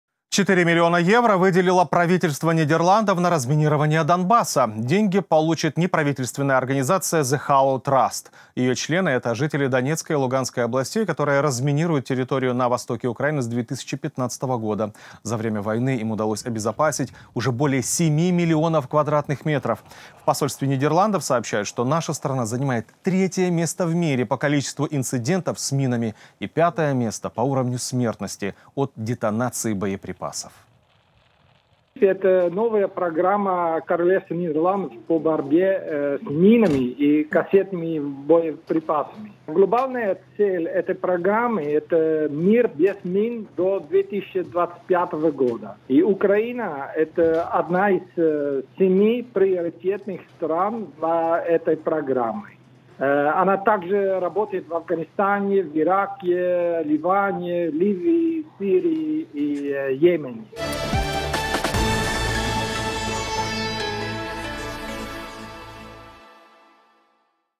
Нидерланды выделили Украине 4 млн евро на разминирование Донбасса
4 млн евро выделило правительство Нидерландов на разминирование Донбасса. Деньги получит неправительственная организация "The HALO Trust". Её члены – жители Донецкой и Луганской областей, которые разминируют территории на востоке Украины с 2015-го года. В Посольстве Нидерландов сообщают, что наша страна занимает 3-е место в мире по количеству инцидентов с минами и 5-е место по уровню смертности от детонации боеприпасов.